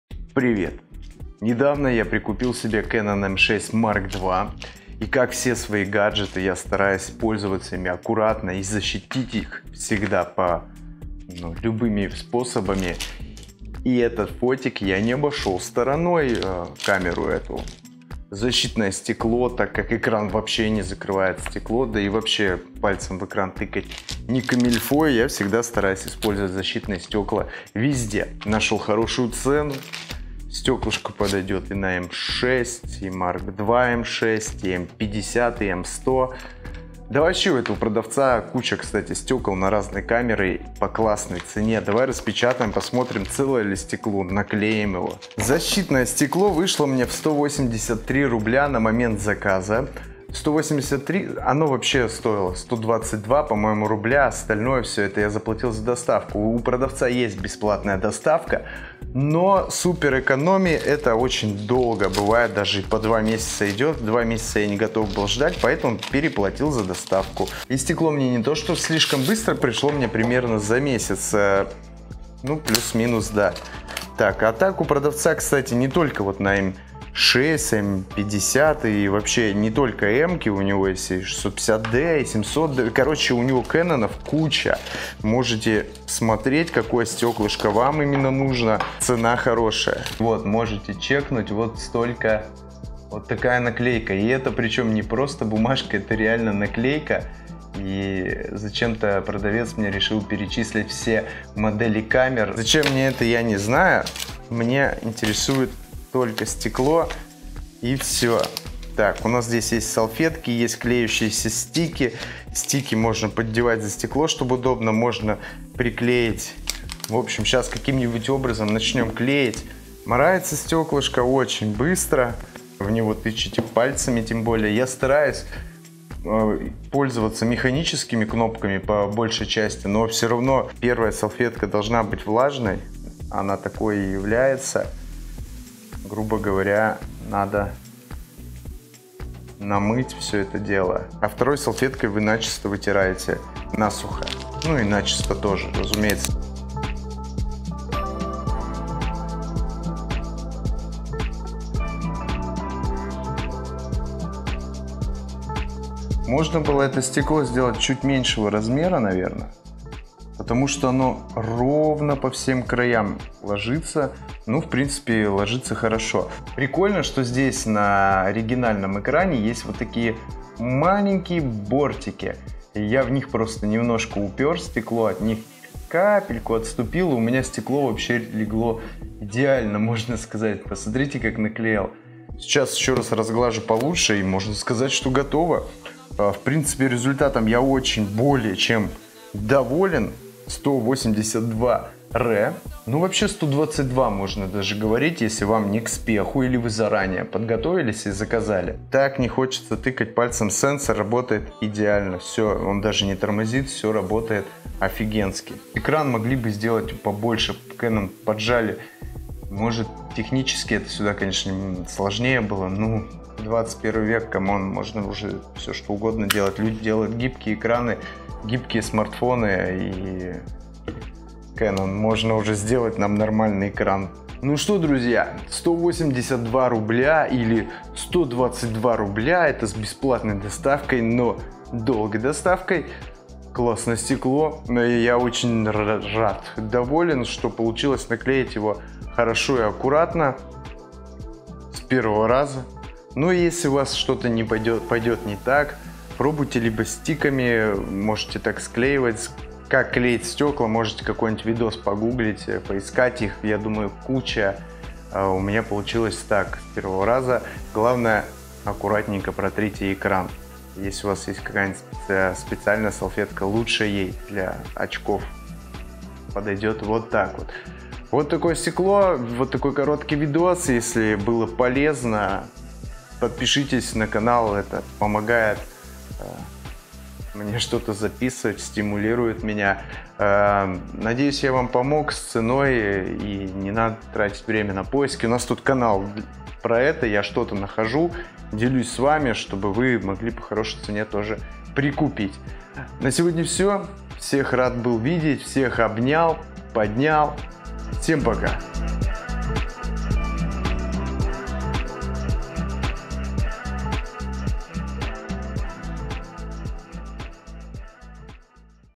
Защитное стекло для Canon EOS M6 Mark II
Ребята ПРИВЕТ!!! Вы на канале Антон Селиванов эт я)) Не так давно я обзавёлся новенькой камерой Canon EOS M6 Mark II. И сразу же начал закупать на него всякие нужные приколюхи. Один из важнейших моментов - это было защита нашего экрана от царапин, сколов и потёртостей. Поэтому я прикупил, точнее заказал защитное стекло. Цену нашёл хорошую, у этого продавца есть стёкла на большое количество моделей камер. Давайте вместе распечатаем посылку, посмотрим и оценим, стоило ли оно своих денег, и нашего времени.